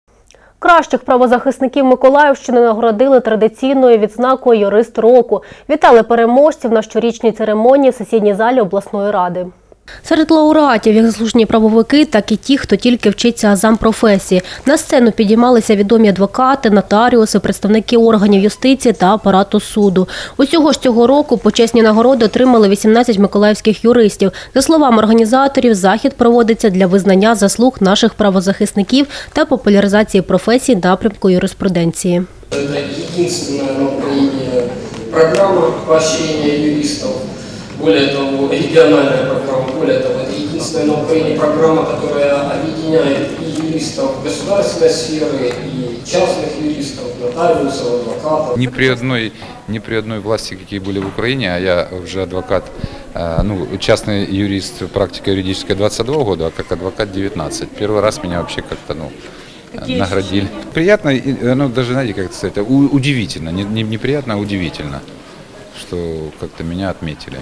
Объектив 17 12 18 В Николаеве наградили лучших юристов года
Автор: Лариса Евсеева
Оператор: Игорь Невидомский